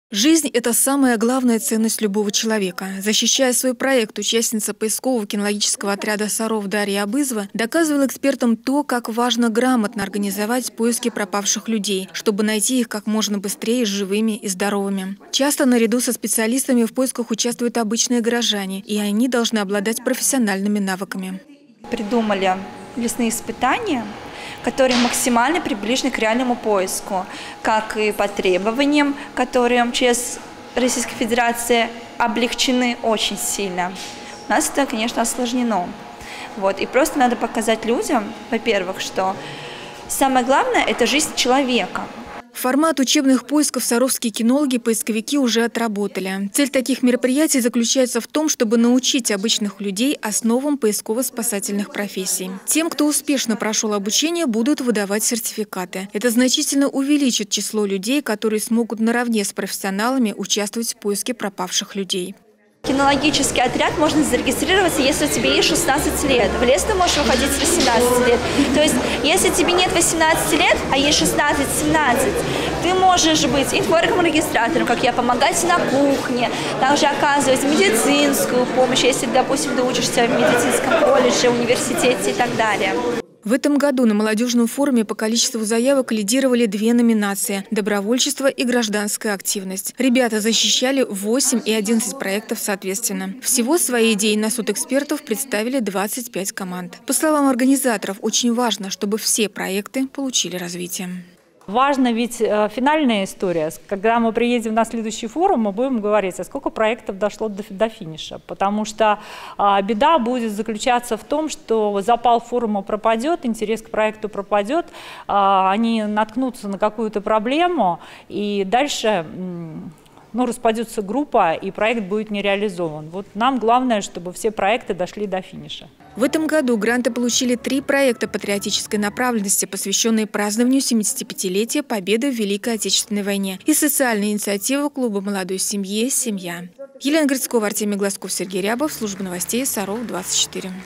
Мой город - моя инициатива
На молодёжном форуме «Время выбрало нас» подвели итоги конкурса социальных проектов «Мой город - моя инициатива».